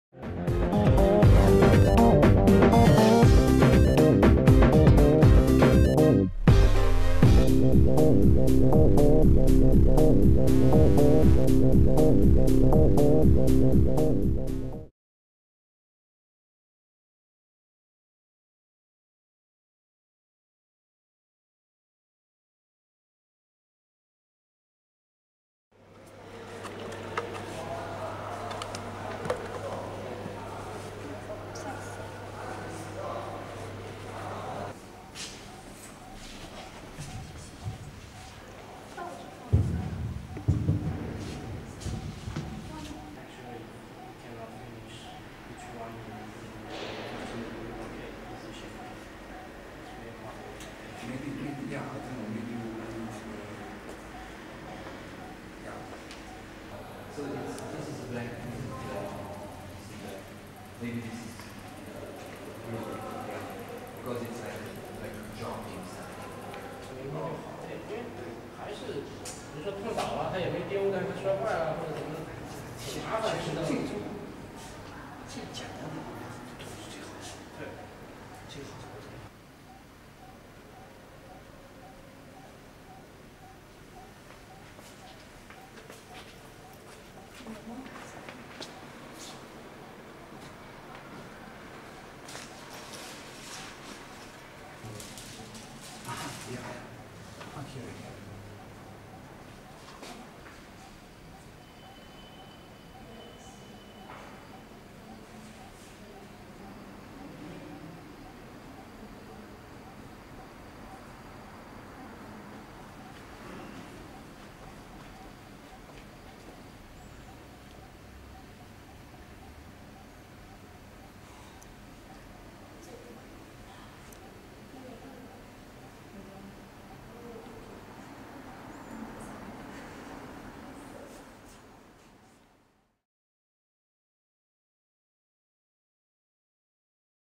用壁画表达建筑的女艺术家 Marjetica Potrč: presenting architecture by murals
Tomaz Tomazin, assistant of Slovenian artist Marjetica Potrč, went to Shenzhen Civic Square on 17 November to prepare for their participating work Mural. Marjeticas researches focus on the forms and types of international architecture and she is fond of presenting the result of her study by means of murals and sculptures. The masterpiece she provides for Shenzhen & Hongkong Bi-city Biennale of Urbanism\Architecture is a mural that portrays the all kinds of buildings constructed in New Orleans after being struck by Katrina Typhoon. The mural will be exhibited in the underground space of Shenzhen Civic Square.